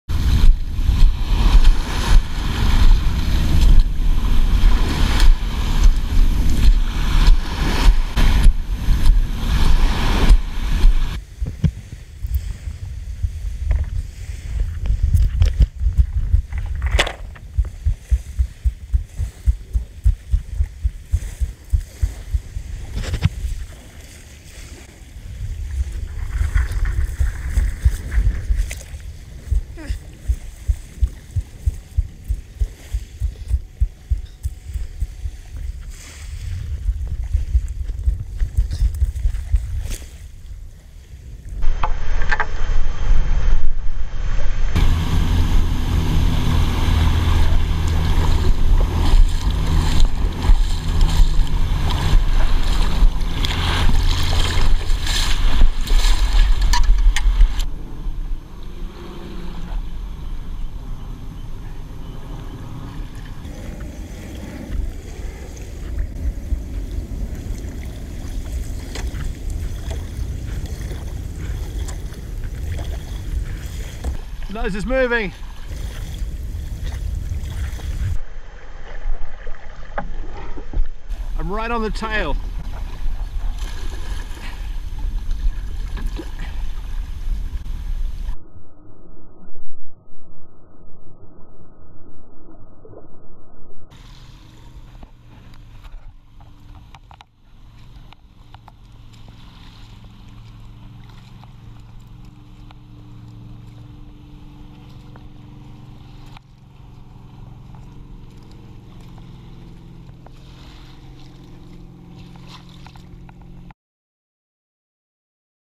Will it SUP? Dragon Boat Piece - Hong Kong
Will it Stand Up Paddle (SUP)? This is a series of vids of items found on the beach that are tested to see if they can be stand up paddled. In this vid, the base of a wooden Dragon Boat is tested... Will it SUP?